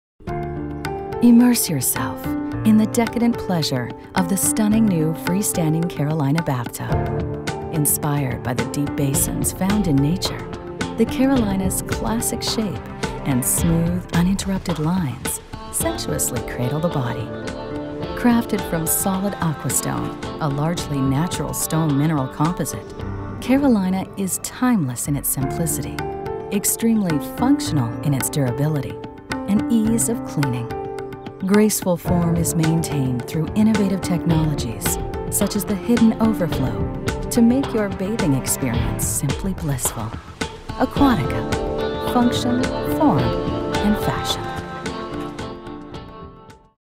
Karolina™ by Aquatica Freestanding Bath Infomercial | KitchenSource.com
The elegant lines and natural stone-like quality of Karolina™ freestanding bathtub by Aquatica creates an instant "I want" factor in any bathroom environment.